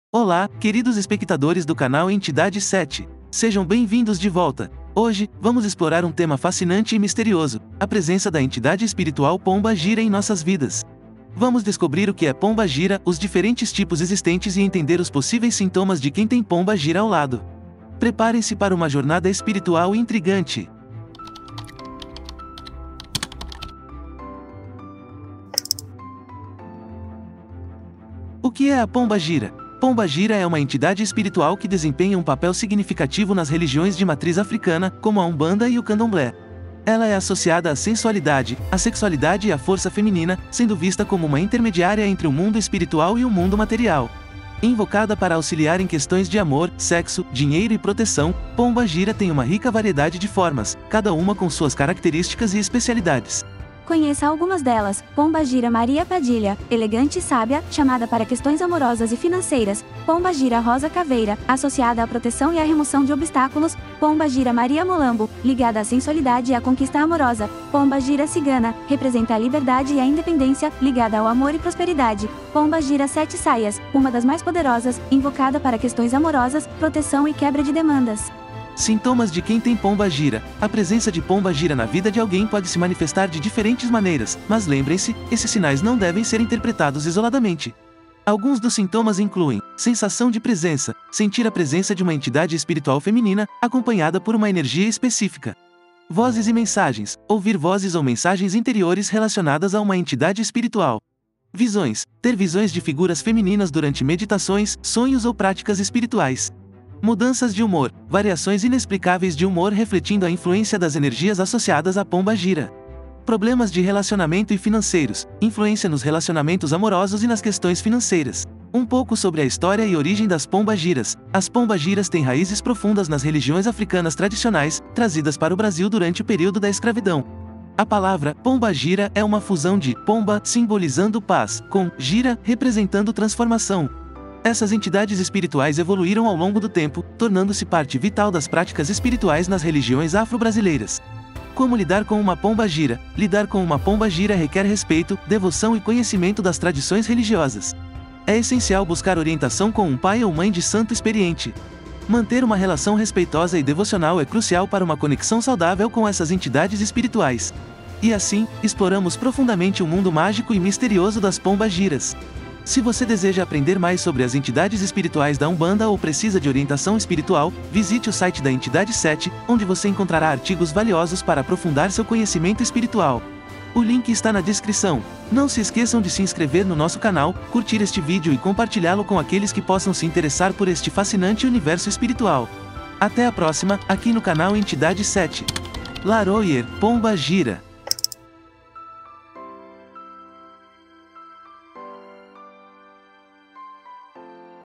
Sintomas de quem tem Pomba Gira ao lado! SAIBA quais são!
Descubra quais são os sintomas de quem tem Pomba Gira ao lado e entenda um pouco mais sobre essa entidade espiritual tão importante nos cultos da Umbanda e outras religiões de matrizes afro brasileiras.